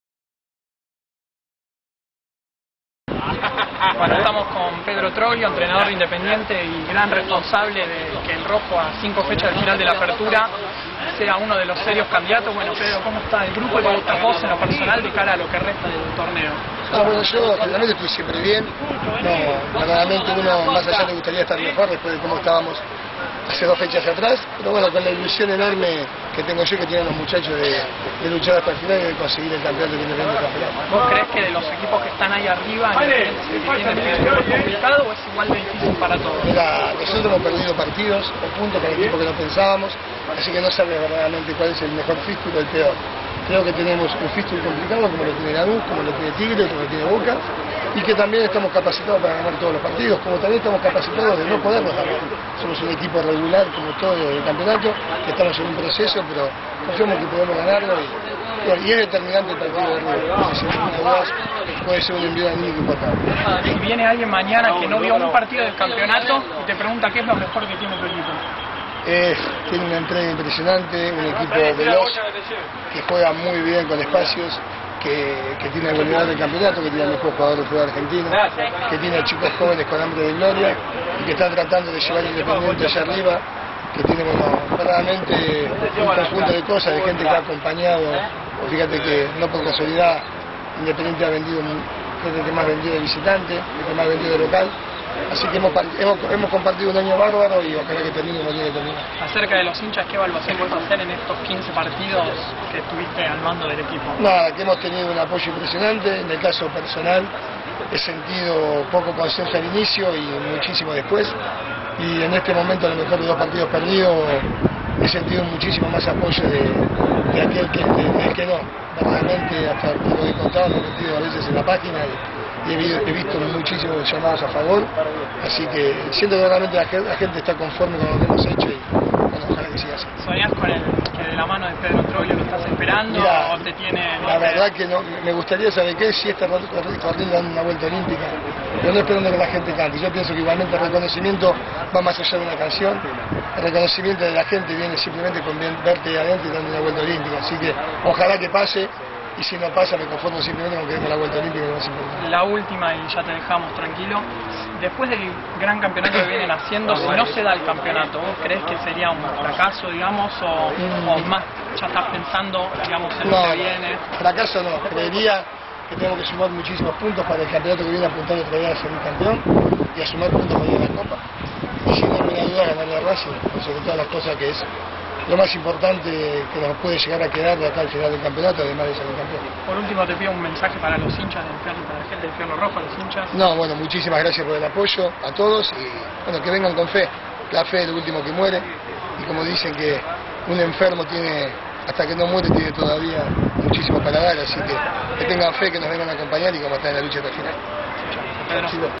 Entrevista Exclusiva Pedro Troglio
Un dia antes del partido decisivo frente a River, Pedro Troglio en una exclusiva para InfiernoRojo.com